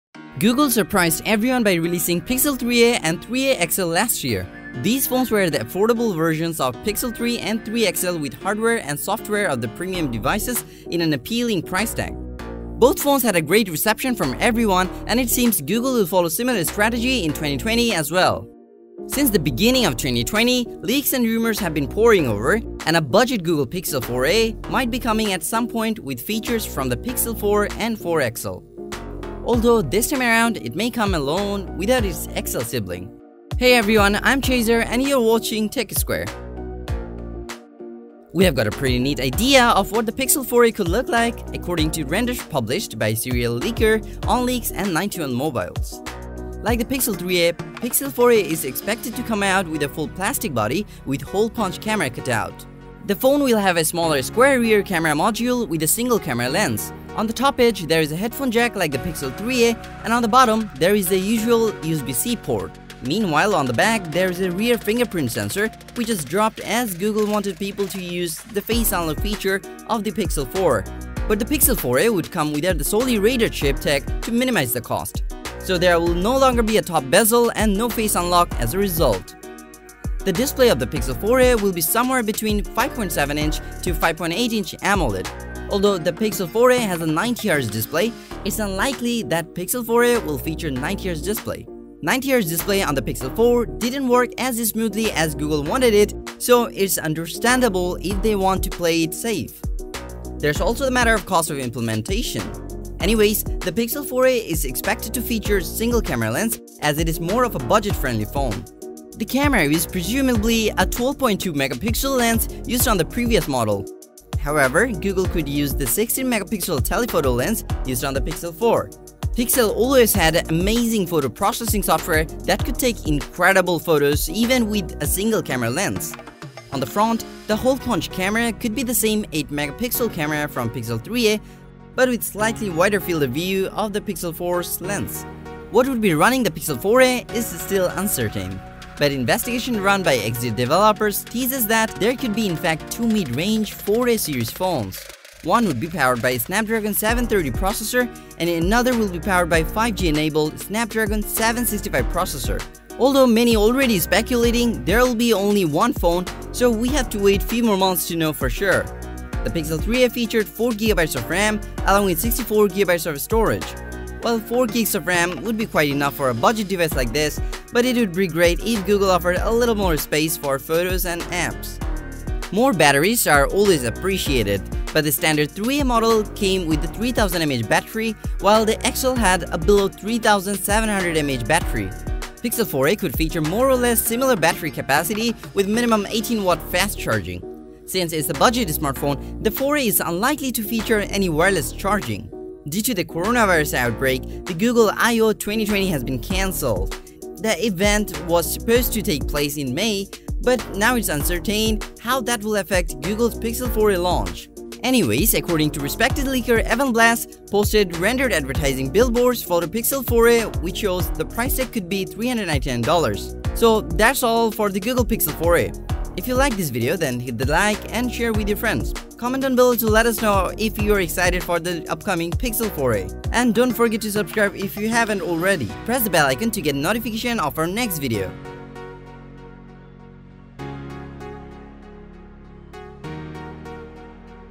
Google Pixel 4a Confirmed Leaks, Rumors & Release Date
In this video, I'm Going to Talk about all the Confirmed Leaks, Rumors and Release Date of Google Pixel 4a.

Best Pixel 4 Deals on Amazon